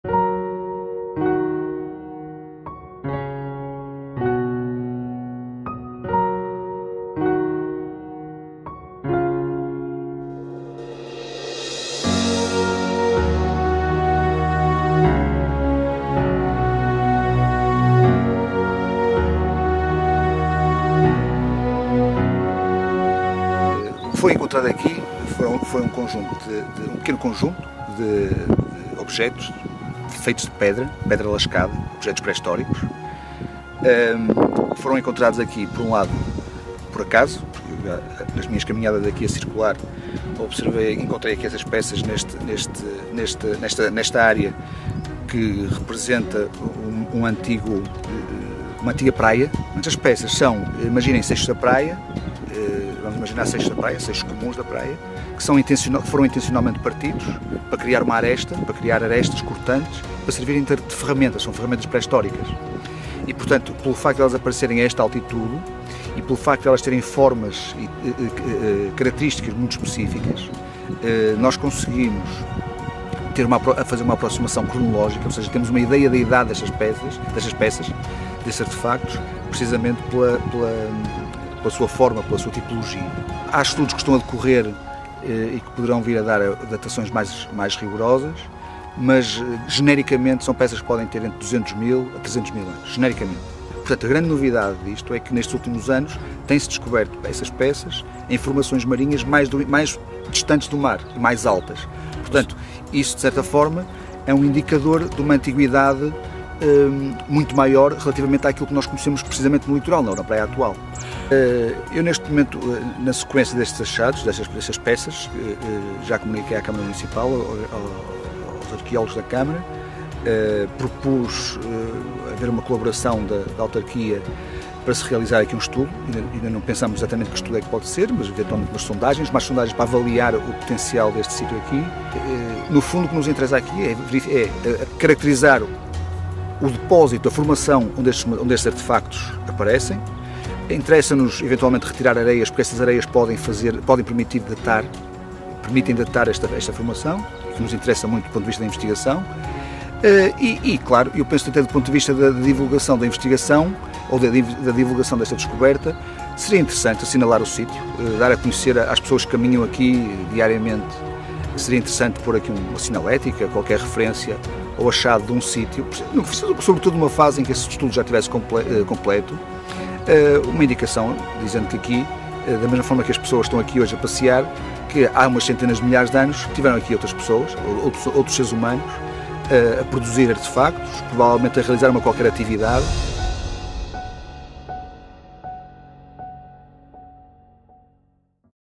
Artefactos achados no canal de Esposende
Era um simples passeio do arqueólogo e docente da UPorto.  Mas o olhar apurado de Sérgio Monteiro Rodrigues terminou num achado de artefactos pré-históricos no canal de Esposende